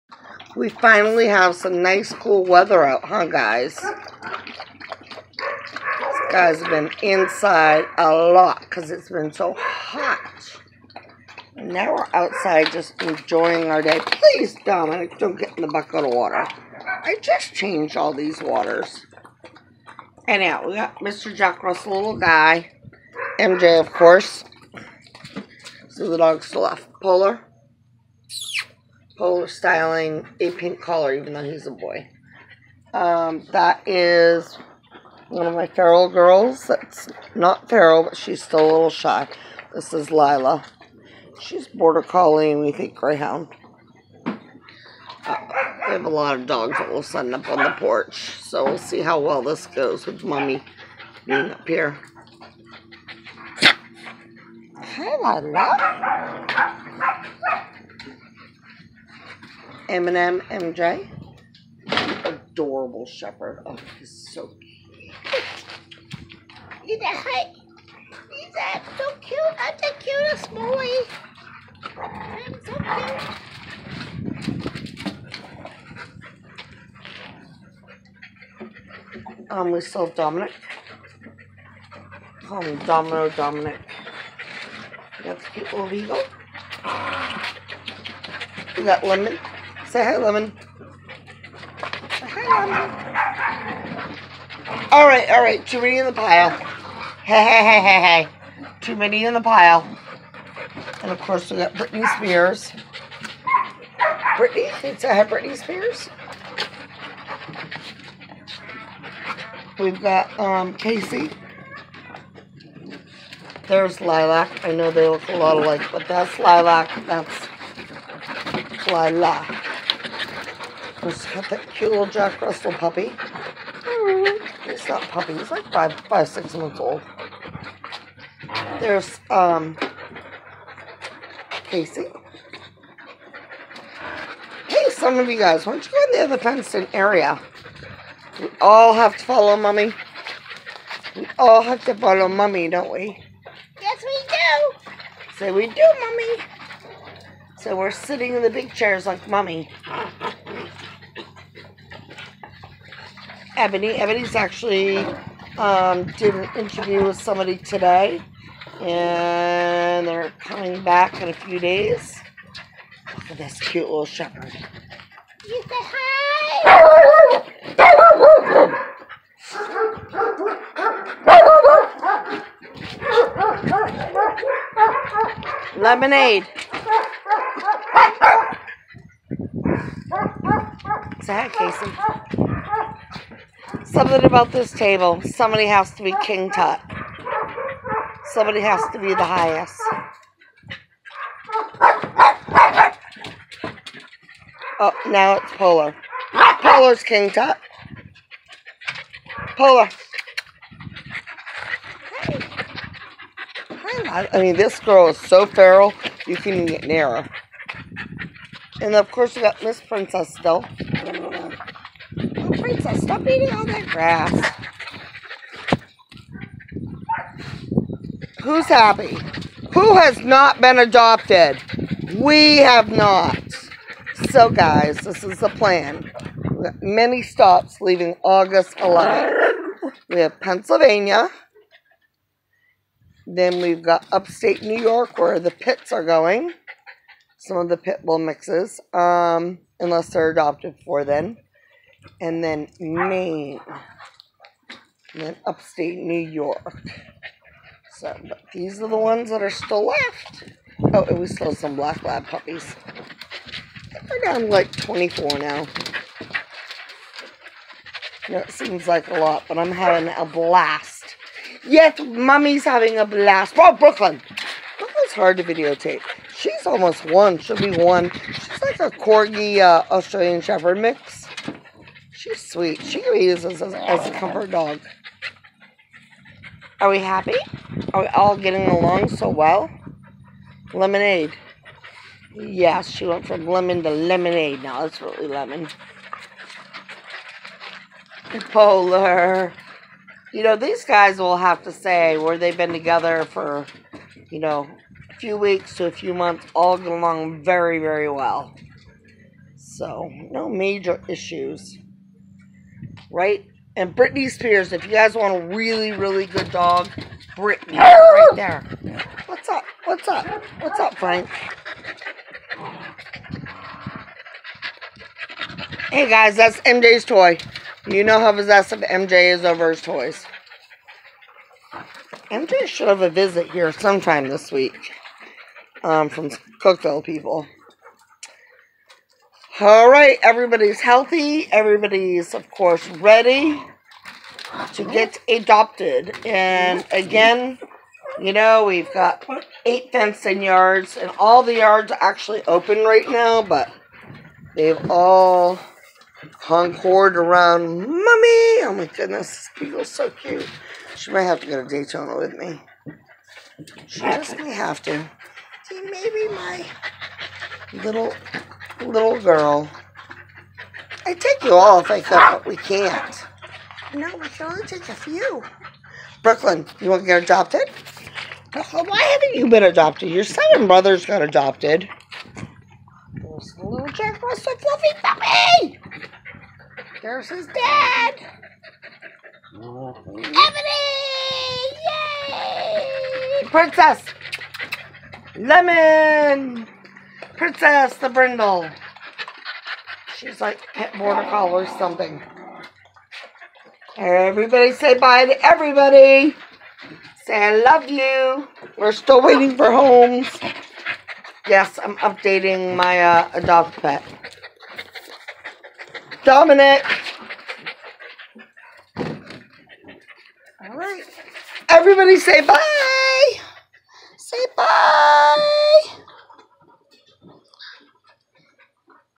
July and Aug Available 2019 Rescue Dog Pup To Adopt
Hi y'all as many of you know I am moving to Daytona Beach after rescuing dogs for 10 years in TN. These are many of the remaining dogs waiting for homes. Our plan is to come to PA Aug 12th for a couple of approved adoptions all ready approved. So if there is any interest in the above video and the next few videos that I post please call me at Or email us for a application at All dogs are completely vetted all vaccines, dewormed and all are spay and neutered. They will come with Health certificates. Take Care from Paula